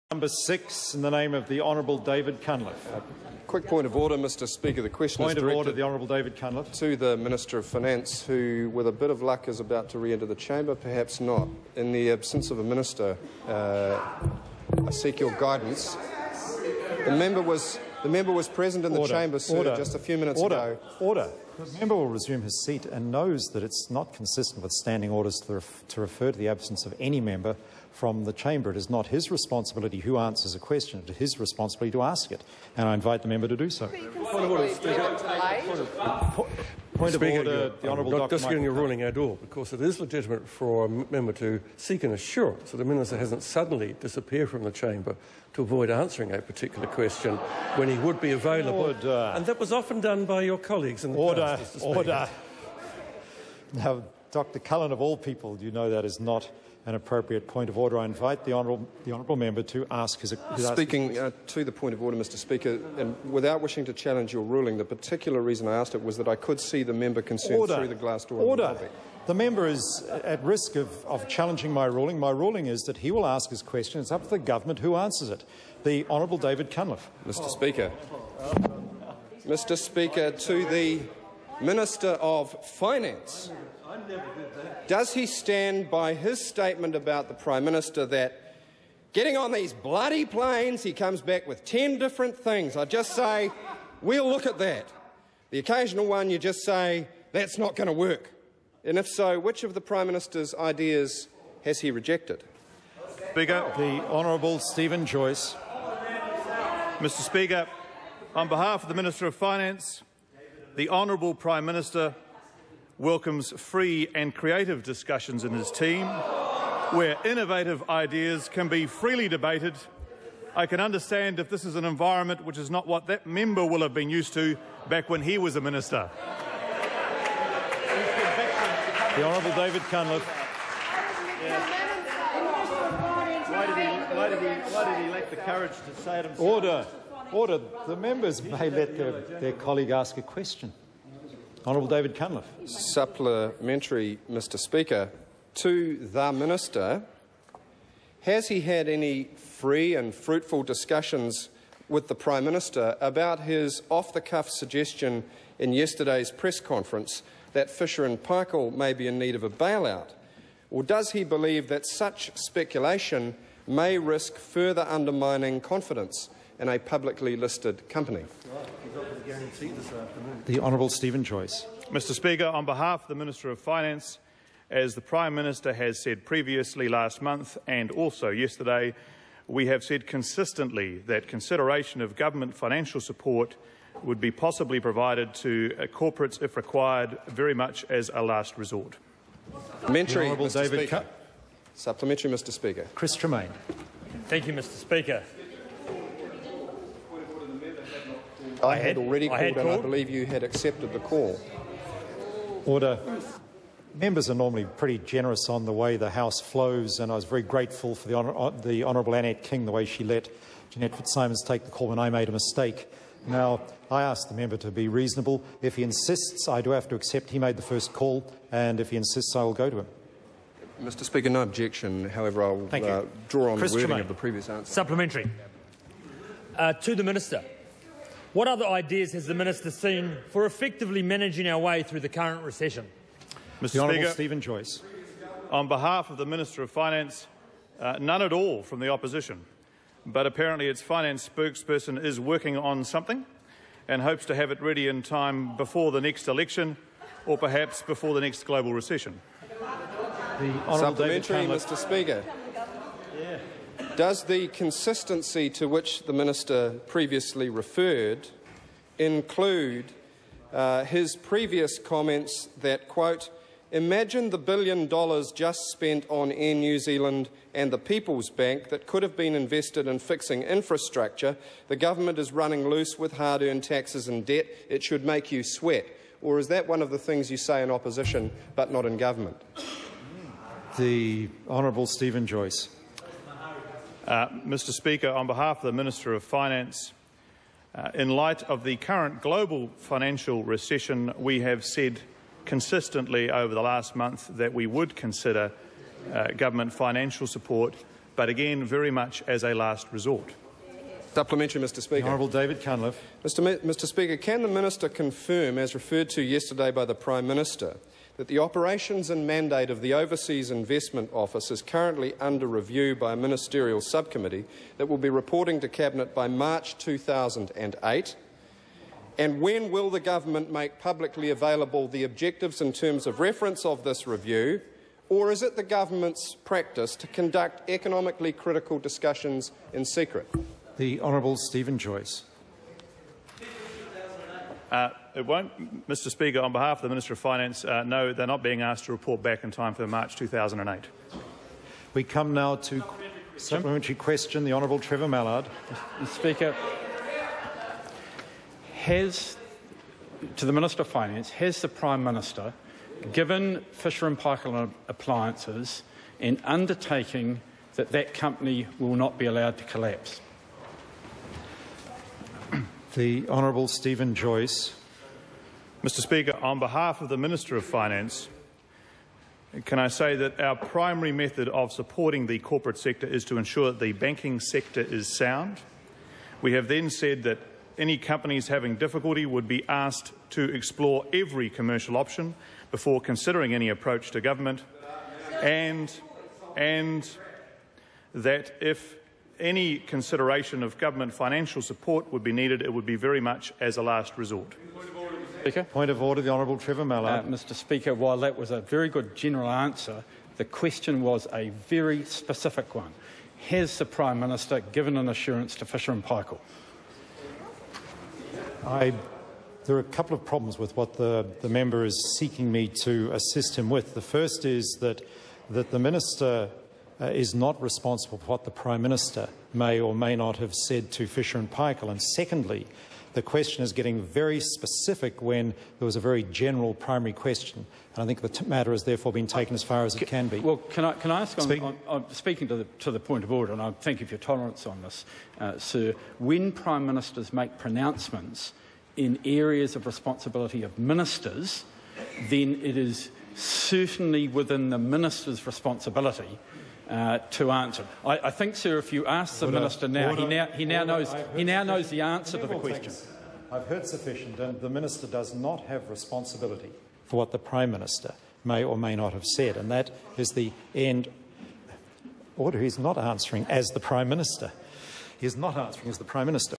Labour attempts to drive wedge between John Key & Bill English
Labour attempting to drive wedge between John Key and BIll English.